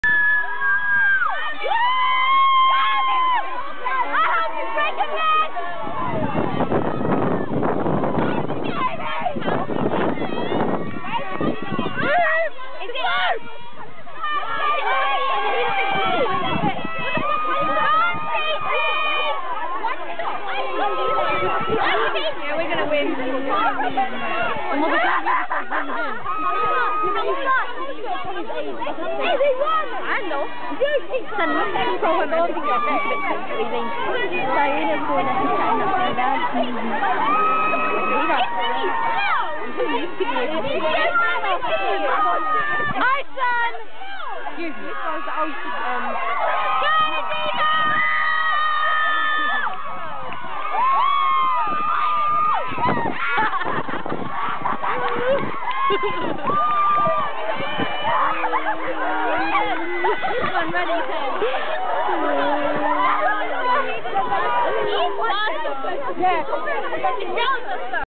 connaught school for girls - teachers v pupils relay race
sports day 2007